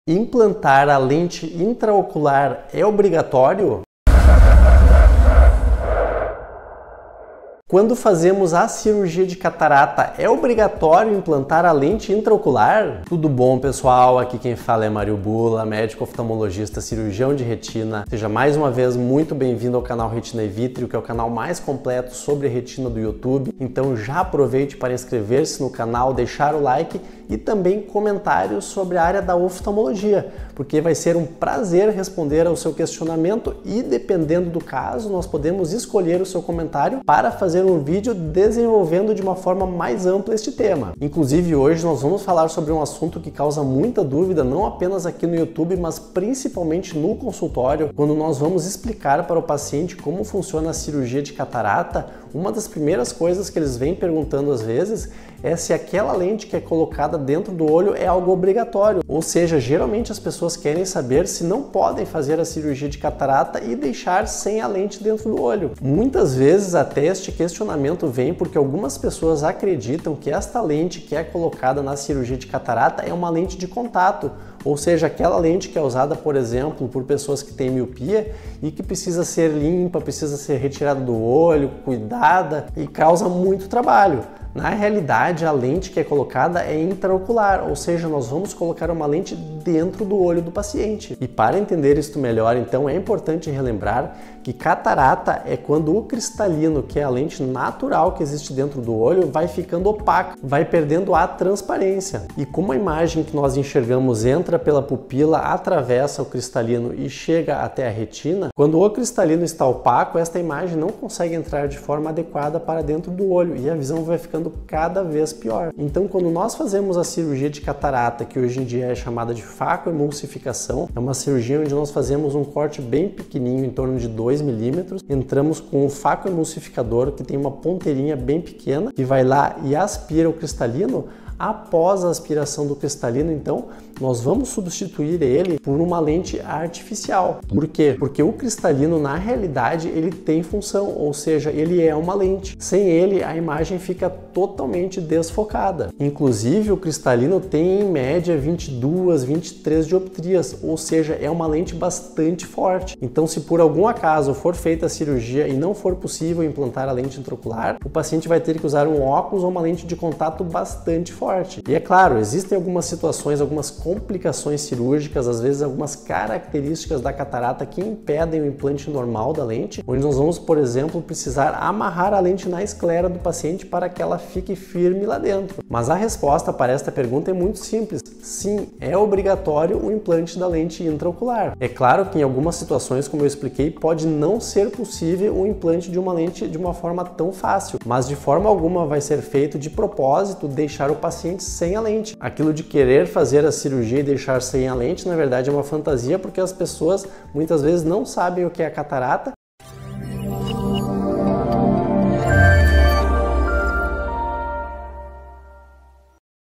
É obrigatório implantar a lente na cirurgia de catarata?
Neste vídeo, o Dr. Mário Bulla, médico oftalmologista, especialista em retina, explica sobre cirurgia de catarata e a necessidade de implantar a lente imtraocular.

A cirurgia de catarata é um procedimento comum que ajuda a restaurar a visão de milhões de pessoas em todo o mundo. Um dos pontos de maior confusão para os pacientes é se a implantação da lente intraocular é obrigatória durante a cirurgia. Neste artigo, vamos esclarecer essa dúvida e explicar por que a lente intraocular é fundamental nesse processo.

O Que é a Cirurgia de Catarata?
Para entender a importância da lente intraocular, primeiro precisamos compreender o que é a catarata. A catarata é uma condição em que a lente natural do olho se torna opaca, prejudicando a visão. Durante a cirurgia de catarata, essa lente opaca é removida e substituída por uma lente artificial, conhecida como lente intraocular.

A Lente Intraocular é Realmente Necessária?
A dúvida que muitos pacientes têm é se é possível realizar a cirurgia de catarata sem a implantação da lente intraocular. A resposta é simples: não. A lente intraocular é fundamental para restaurar a visão após a remoção da lente natural opaca.

Por Que a Lente Intraocular é Essencial?
A lente intraocular desempenha um papel crucial no processo de correção da visão após a cirurgia de catarata. A lente natural do olho tem um poder de correção de cerca de 22 dioptrias, o que a torna fundamental para a visão clara. Sem essa lente, a visão seria significativamente prejudicada, e os pacientes teriam que depender de óculos ou lentes de contato extremamente fortes.

Possíveis Complicações e Soluções
Embora a implantação da lente intraocular seja o procedimento padrão durante a cirurgia de catarata, em alguns casos, podem ocorrer complicações que tornam essa etapa mais desafiadora. Por exemplo, em situações em que a cápsula anterior da lente está comprometida, pode ser necessário fixar a lente intraocular à esclera do paciente para garantir sua estabilidade.

Conclusão
Em resumo, a resposta à pergunta "a implantação da lente intraocular é obrigatória na cirurgia de catarata?" é definitivamente sim. A lente intraocular desempenha um papel vital na restauração da visão após a remoção da lente opaca durante a cirurgia de catarata. Sem ela, a visão permaneceria prejudicada, e os pacientes enfrentariam dificuldades significativas.

Esperamos que este artigo tenha esclarecido qualquer dúvida que você pudesse ter sobre a importância da lente intraocular na cirurgia de catarata.

Perguntas Frequentes
Posso fazer a cirurgia de catarata sem a implantação da lente intraocular?

Não, a lente intraocular é fundamental para restaurar a visão após a cirurgia de catarata.
A lente intraocular é permanente?

Sim, a lente intraocular é projetada para ser permanente e não precisa ser substituída.
Existem diferentes tipos de lentes intraoculares?

Sim, há vários tipos de lentes intraoculares disponíveis, cada uma com características específicas.
Quanto tempo leva para a recuperação após a cirurgia de catarata?

A recuperação varia de pessoa para pessoa, mas geralmente leva algumas semanas para uma visão clara e estável.
A cirurgia de catarata é segura?

Sim, a cirurgia de catarata é considerada segura e eficaz na maioria dos casos, com baixo risco de complicações.

Autor:
Dr. Mário César Bulla
Cremers 28120
Médico Oftalmologista - Retinólogo

0:00 Implantar a lente intraocular é obrigatorio?
1:20 A lente intraocular.
1:30 O que é catarata?
1:56 A cirurgia de catarata.
2:18 Por que implantar a lente?